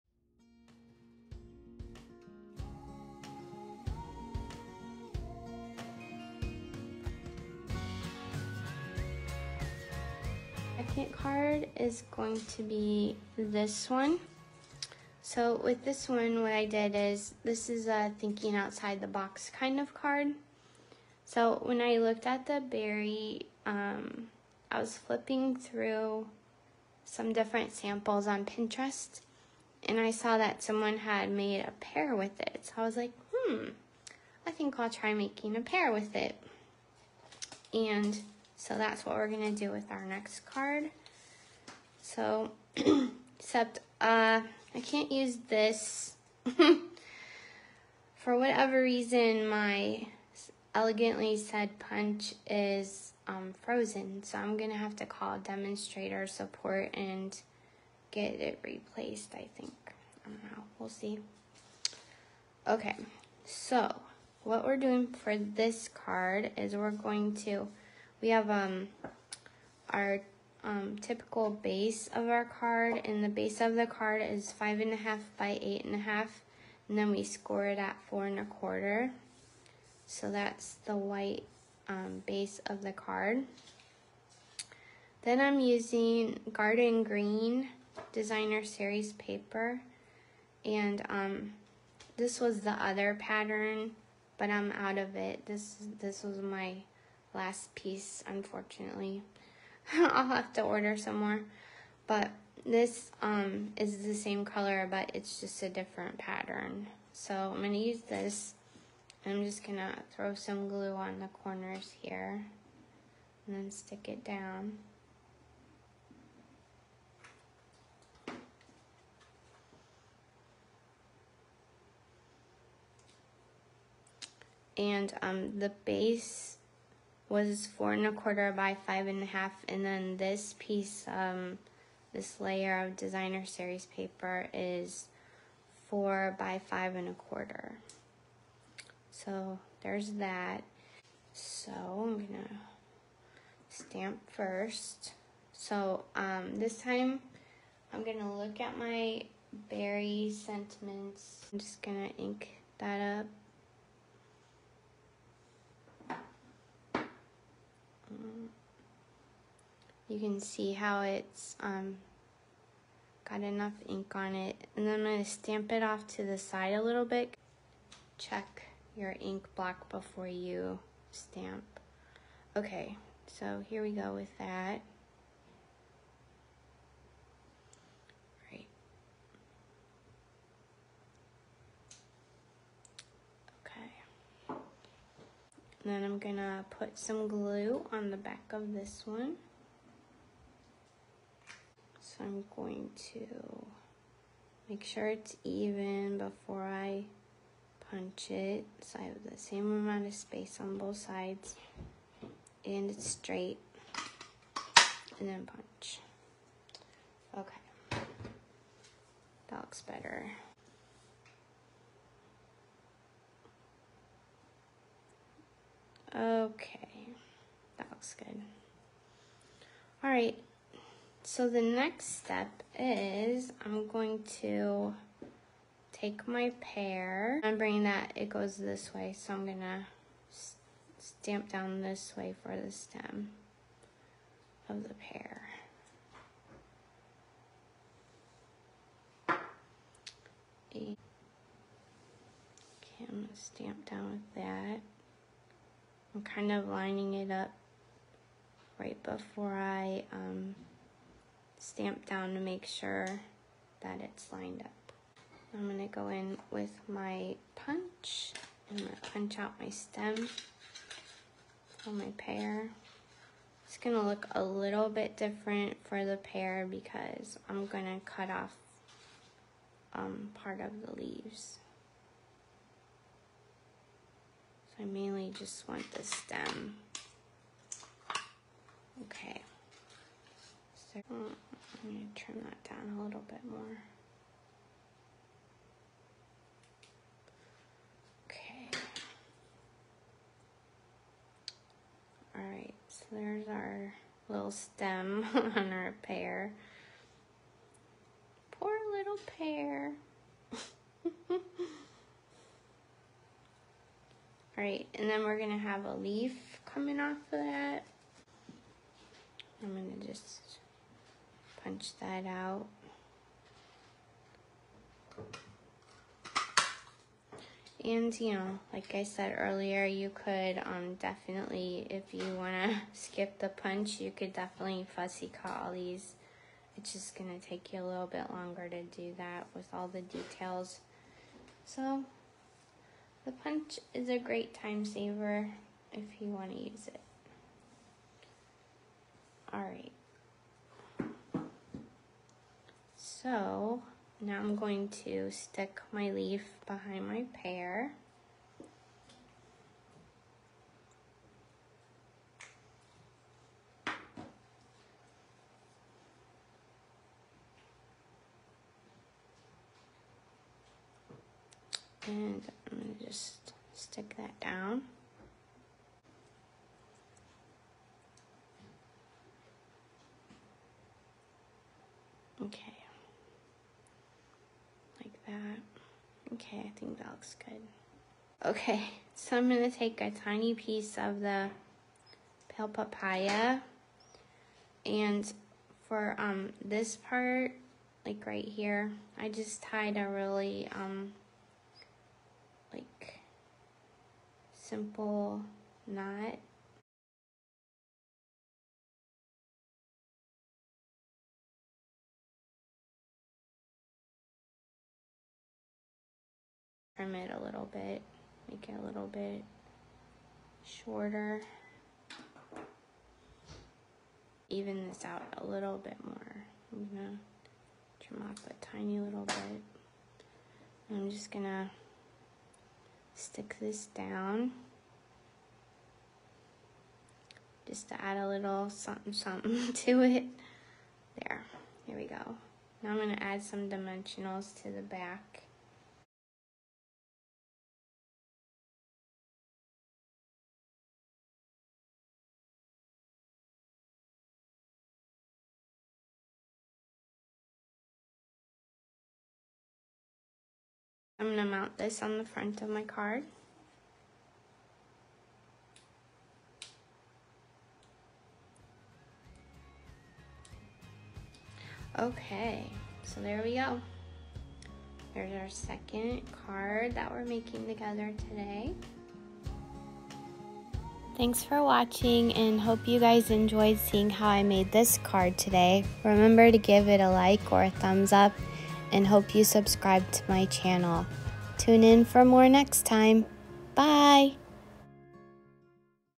Thank you Peary Much!
Using the strawberry builder punch to create a pear instead of a strawberry.